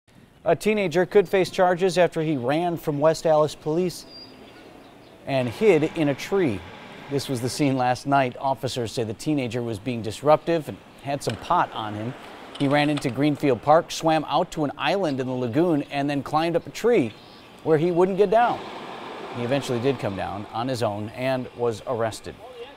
Teen hides from West Allis Police in tree
A 15-year-old boy was arrested in West Allis after he ran from police and hid in a tree in Greenfield Park.